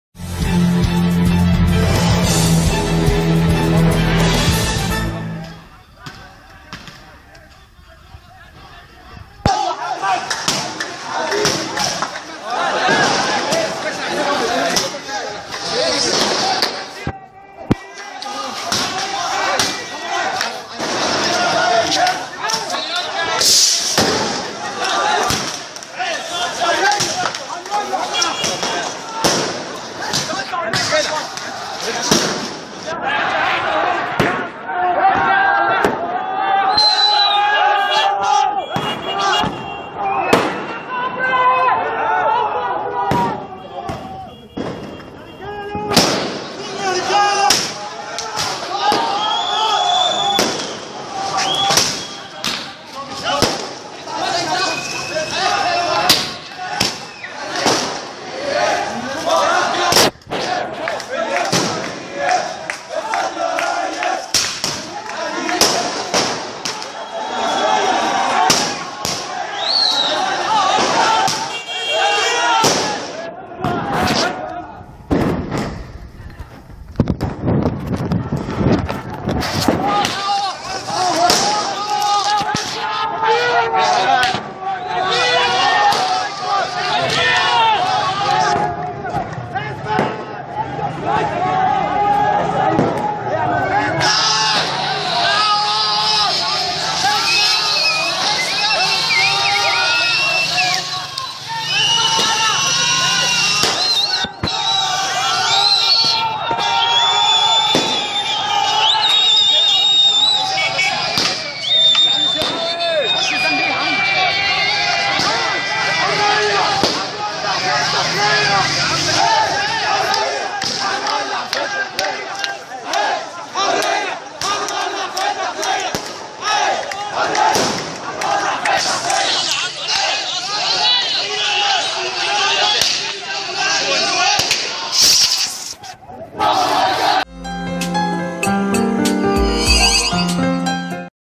اشتباكات بين قوات الامن و الثوار بالزقازيق 7/3 من امام منزل الدكتور محمد مرسى
اشتباكات بين قوات الامن و الثوار بالزقازيق 7/3 من امام منزل الدكتور محمد مرسى - الشرقية